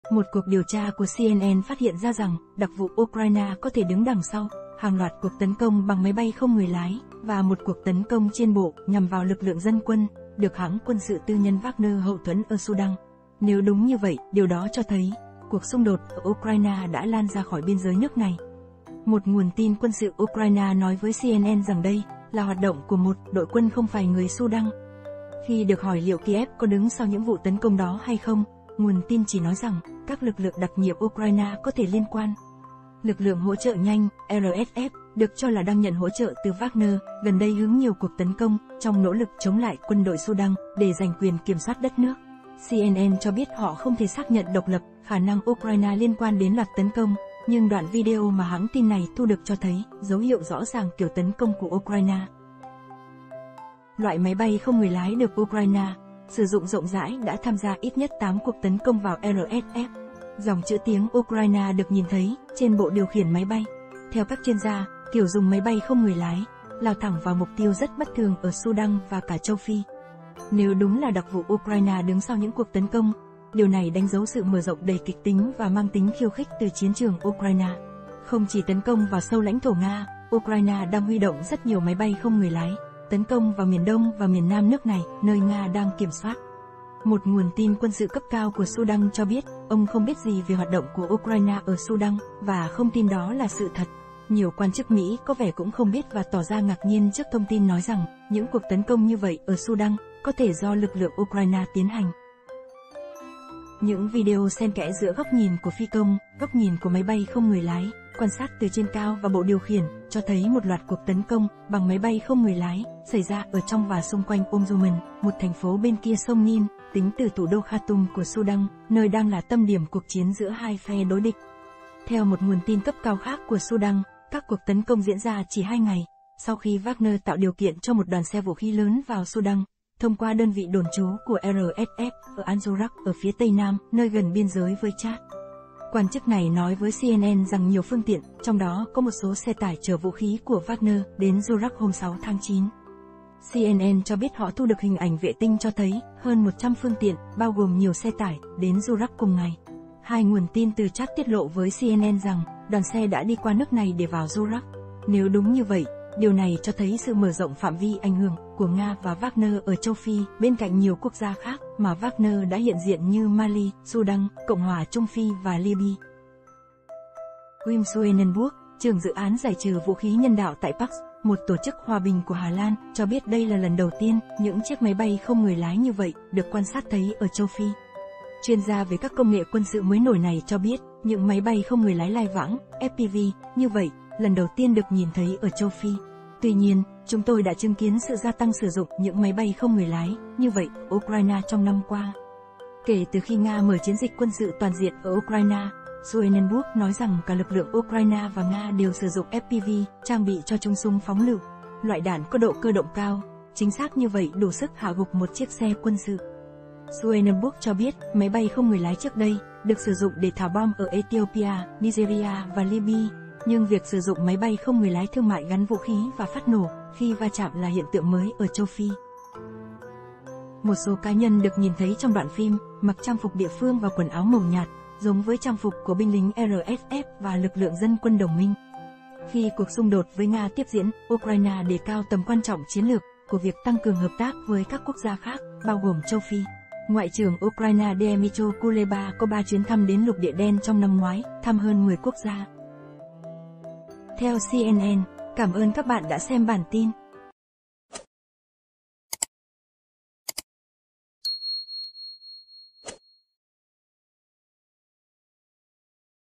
ĐẶC VỤ UKRAINE CÓ THỂ ĐỨNG SAU LOẠT TẤN CÔNG BẰNG MÁY BAY KHÔNG NGƯỜI LÁI Ở SUDAN
Một cuộc điều tra của CNN phát hiện ra rằng, đặc vụ Ukraine có thể đứng đằng sau, hàng loạt cuộc tấn công bằng máy bay không người lái, và một cuộc tấn công trên bộ, nhằm vào lực lượng dân quân, được hãng quân sự tư nhân Wagner hậu thuẫn ở Sudan. Nếu đúng như vậy, điều đó cho thấy, cuộc xung đột ở Ukraine đã lan ra khỏi biên giới nước này.